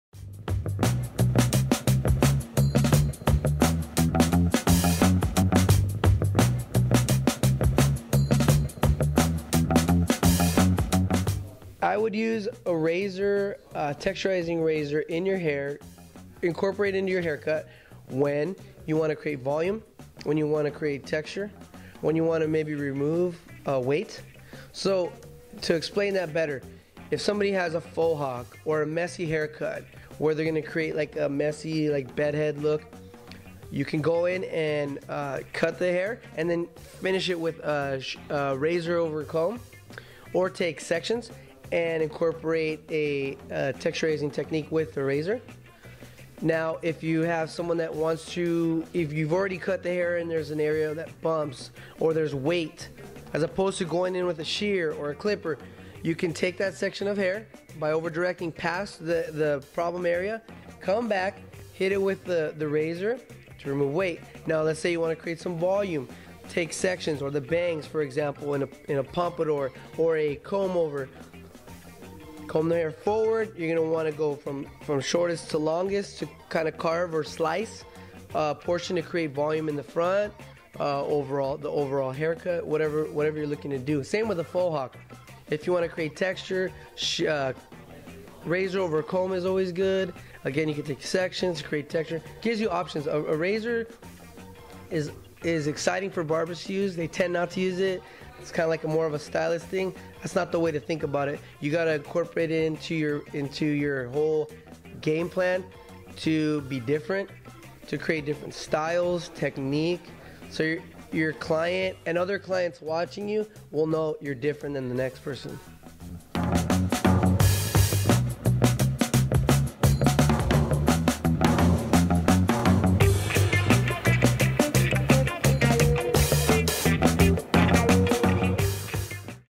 As a Barber, Why Use a Razor in the Hair?
When would you use a razor in the hair? There are several reasons. Here Gabe Luevano, a barber who uses Feather Razors to do hair razor cutting, explains when he uses a razor to achieve the looks he's going for.